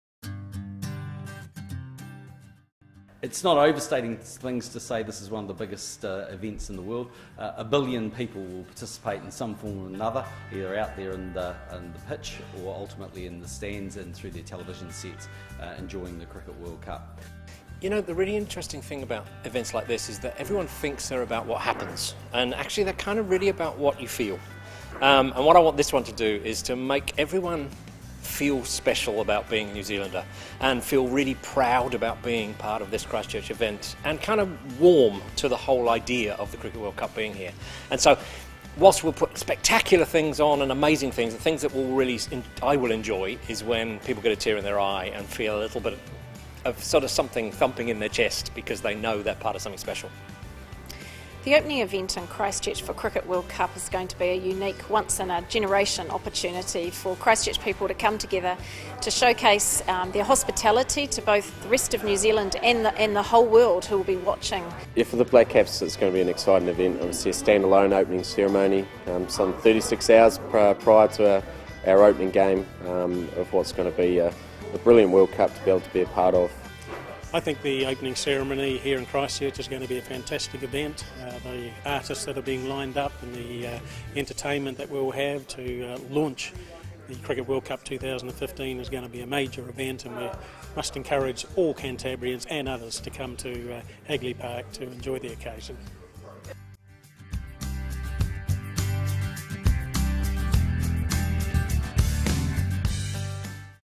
Opening events announced for the ICC Cricket World Cup 2015
The ICC Cricket World Cup 2015 will launch in spectacular fashion with opening events in both Melbourne and Christchurch on 12 February 2015. Both events will be free and open to the public.